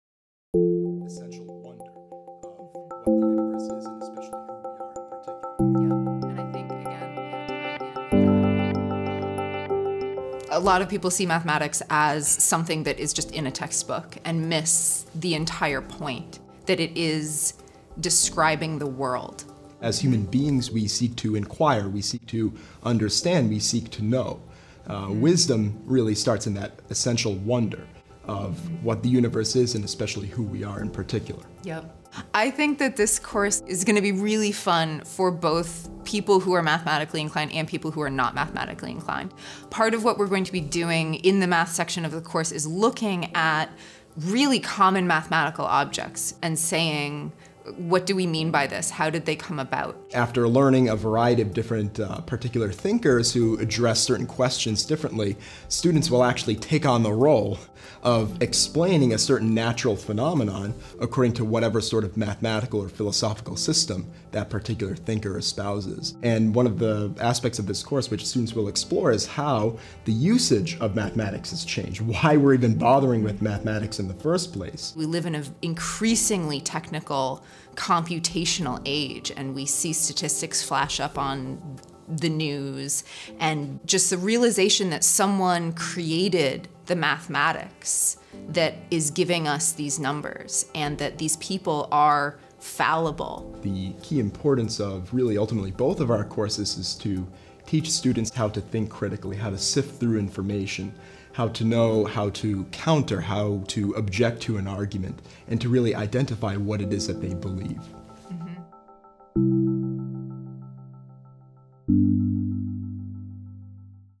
Being Human: Philosophy of Nature and Mathematical Knowledge & Philosophy Understanding Mathematics
Being Human: The Philosophical Problem of Nature and Mathematical Knowledge  & Philosophy Understanding Mathematics: Its Philosophical Origins, Evolution, and Humanity
PHIL170501 + MATH170101
Colin Connors,  Philosophy
Ellen Goldstein, Mathematics
1 Philosophy I + 1 Mathematics
MWF 12–12:50 + MWF 1–1:50
Thurs. 6–7:50 reserved for reflection sessions (4 times during the semester)
How do you explore the world around you? What tools do you use to expand and describe your knowledge of what is real and what is truth? By studying the parallel development of philosophy and mathematics, we will attempt to understand the shifting relationship between humans and nature. These courses will begin with the Greeks’ use of numbers to understand nature and connect to the divine. They will progress to modern uses of mathematics to predict natural events and quantify human observation.
To register for these courses:
Register for both PHIL170501 and MATH170101
Students will automatically be enrolled in PHIL1706 in the spring to complete their Philosophy Core requirement.